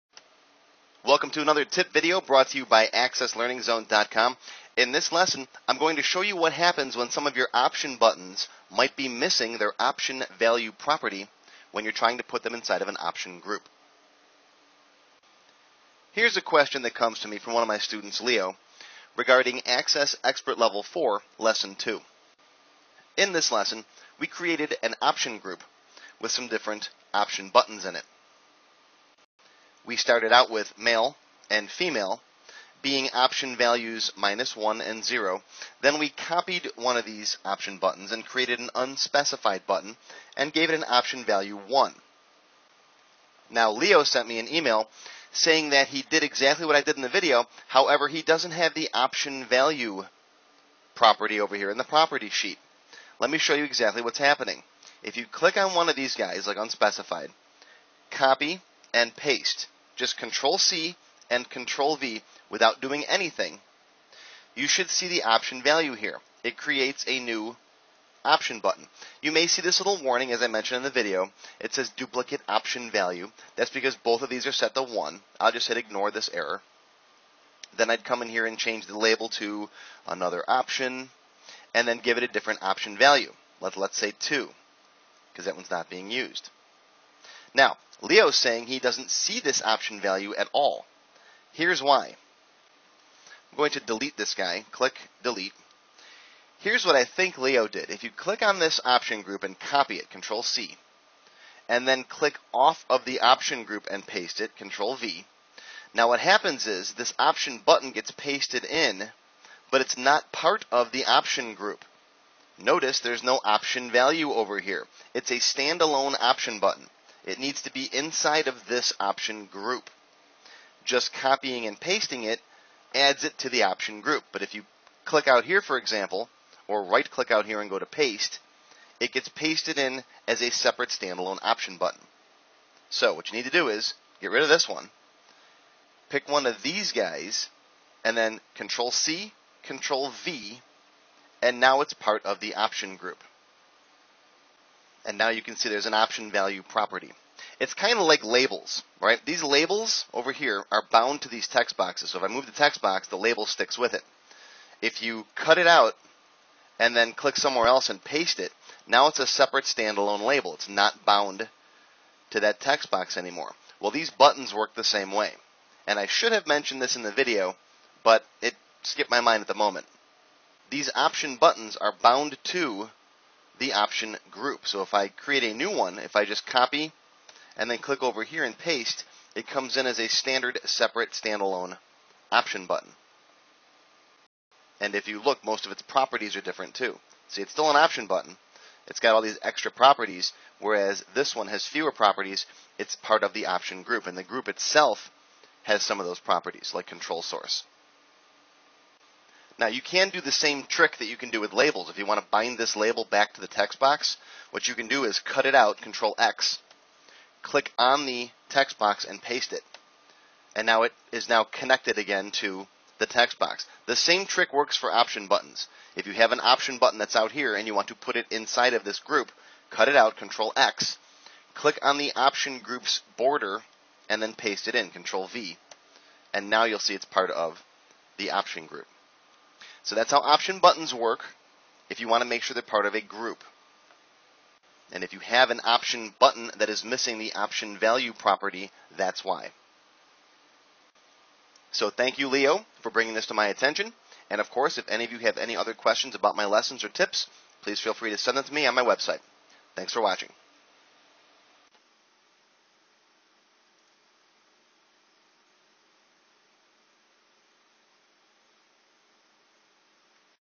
Microsoft Access Option Buttons Missing Option Value Property
In this tutorial I will explain why your Option Buttons may sometimes be missing their Option Value properties when you try to put them in an Option Group in your Microsoft Access databases. Learn more about Option Buttons and Groups